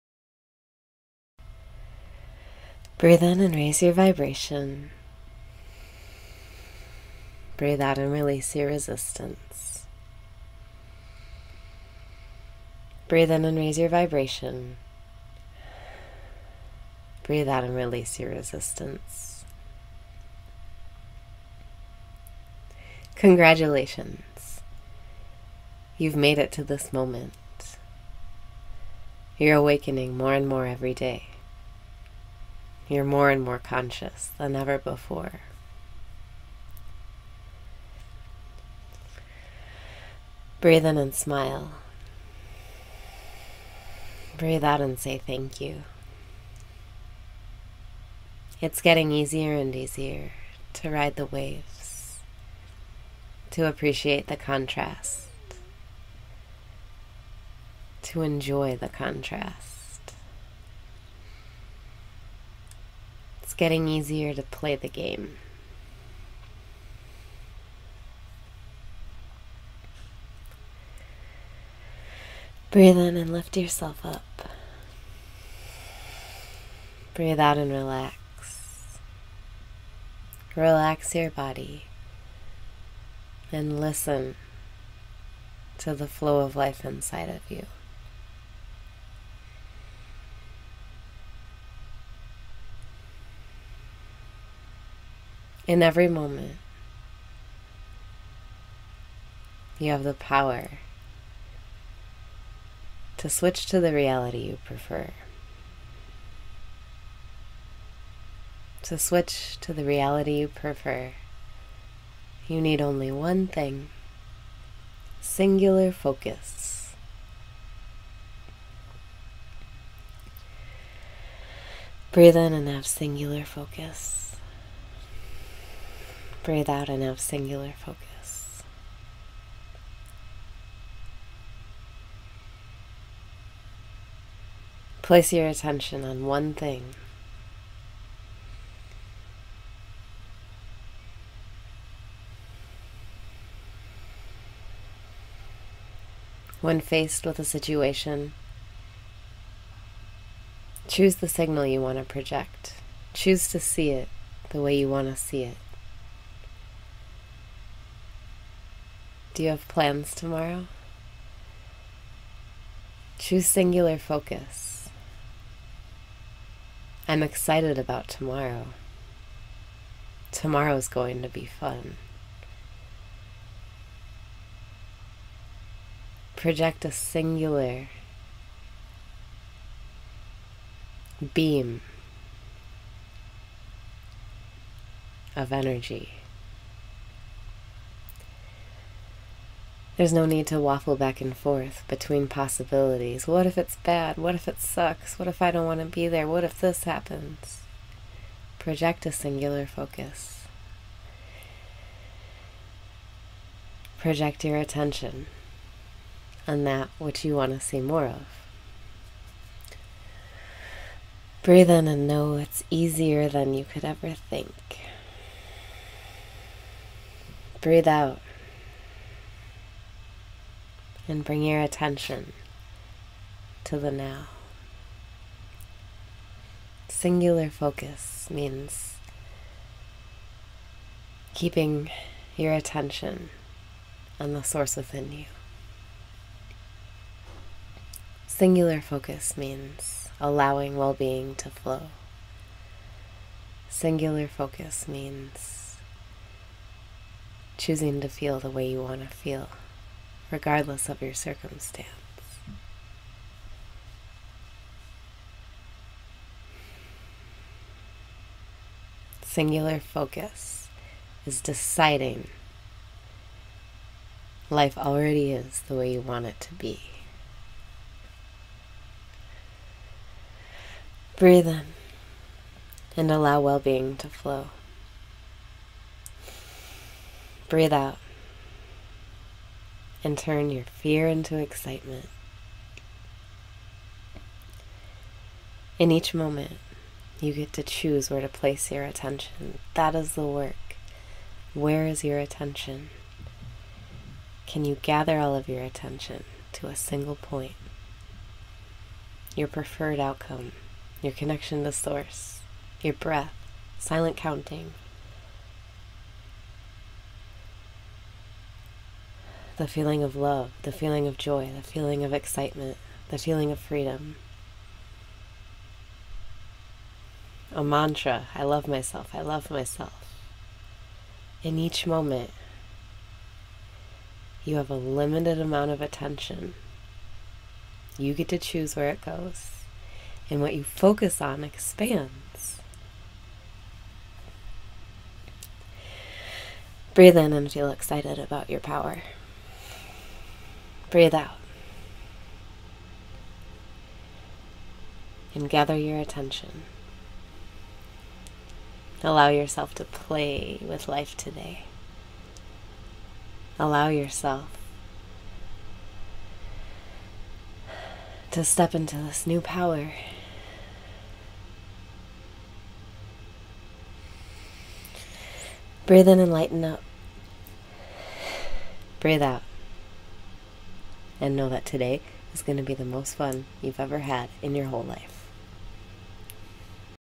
Daily Meditation - Your True Work
Congratulations! You've made it to this moment. You're awakening more and more every day. You're more and more conscious than ever before.

It's getting easier and easier to ride the waves. To appreciate the contrast. To enjoy the contrast. It's getting easier to play the game.

Relax your body. Listen to flow of life inside of you.

In every moment, you have the power to switch to the reality you prefer. To switch to the reality you prefer, you need only one thing: singular focus.

Breathe in and have singular focus.
Breathe out and have singular focus.

Place your attention on one thing. When faced with a situation, choose the signal you want to project. Choose to see it the way you want to see it. Do you have plans tomorrow? Choose singular focus. "I'm excited about tomorrow. Tomorrow's going to be fun." Project a singular beam of energy. There's no need to waffle back and forth between possibilities: "What if it's bad? What if it sucks? What if I don't want to be there? What if this happens?" Project a singular focus. Project your attention on that which you want to see more of.

Breathe in and know it's easier than you could ever think.
Breathe out and bring your attention to the now.

Singular focus means keeping your attention on the source within you. Singular focus means allowing well being to flow. Singular focus means choosing to feel the way you want to feel regardless of your circumstance. Singular focus is deciding life already is the way you want it to be.

Breathe in and allow well being to flow.
Breathe out and turn your fear into excitement.

In each moment, you get to choose where to place your attention. THAT is the work! Where is your attention? Can you gather all your attention to a single point?

Breathe in and feel excited about your power!
Breathe out and gather your attention.

Allow yourself to play with life today. Allow yourself to step into this new power.

Breathe out and know that today is going to be the most fun that you've ever had in your whole life!